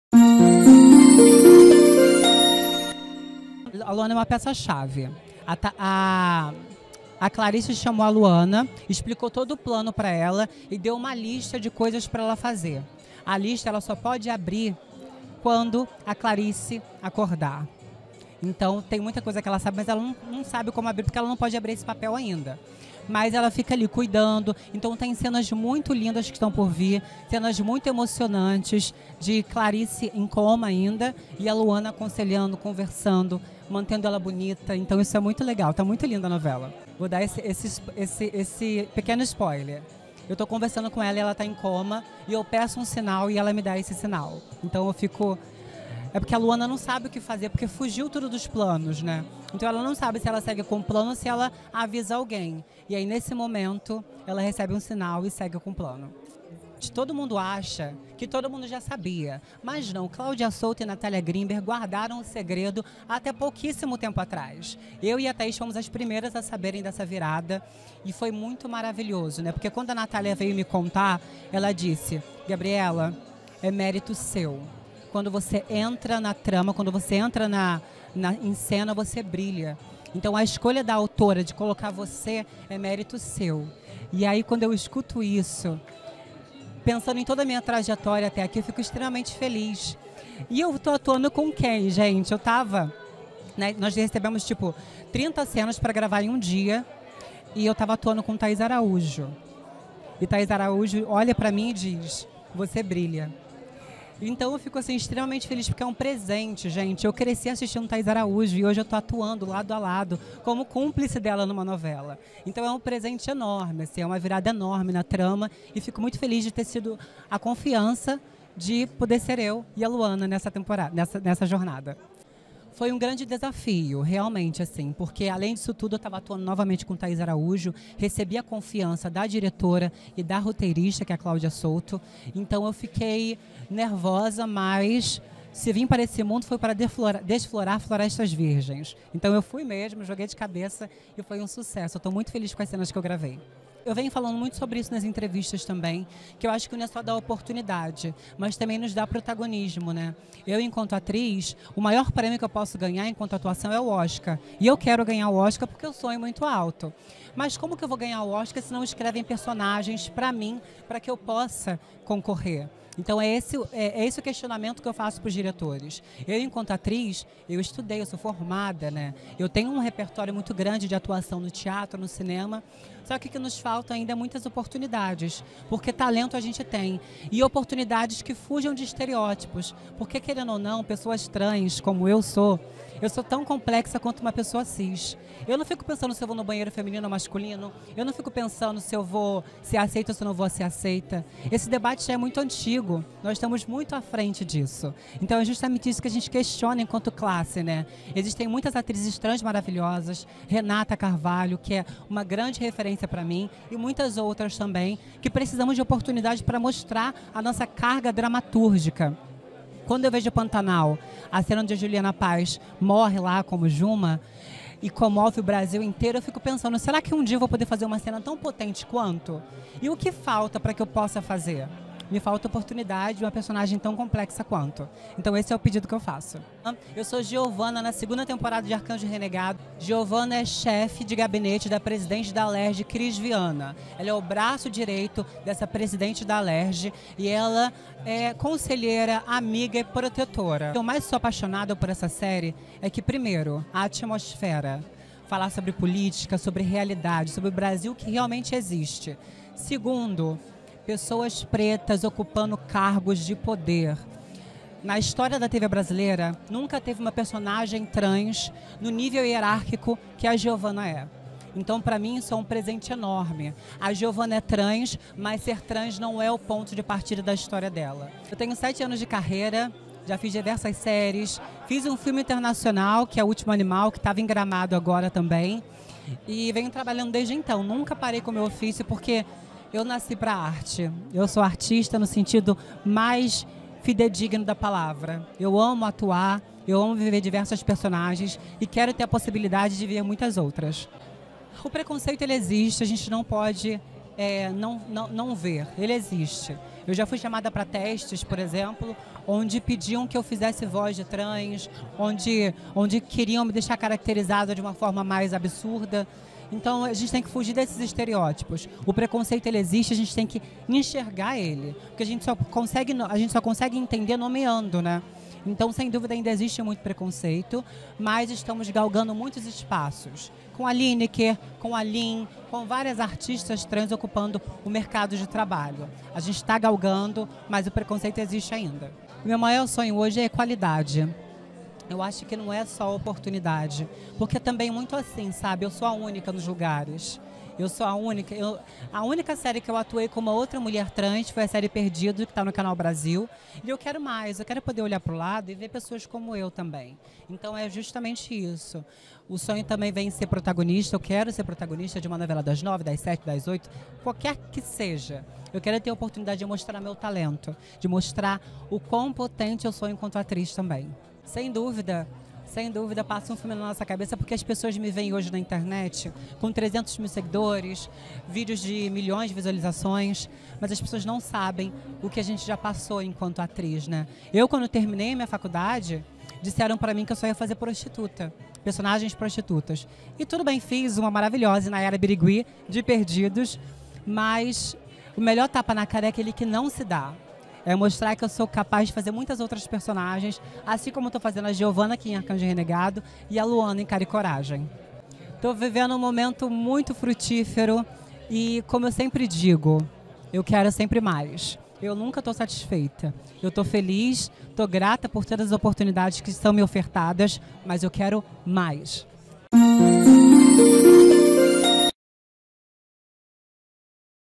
Mulher trans, Gabriela Loran dribla preconceito com talento como a Luana de Cara e Coragem
Veja no ArteBlitz.com - Personagem vira peça-chave em virada da trama das 7 e atriz quer mais espaço na carreira para mostrar sua carga dramatúrgica. "Já ouvi que só faria papel de prostituta".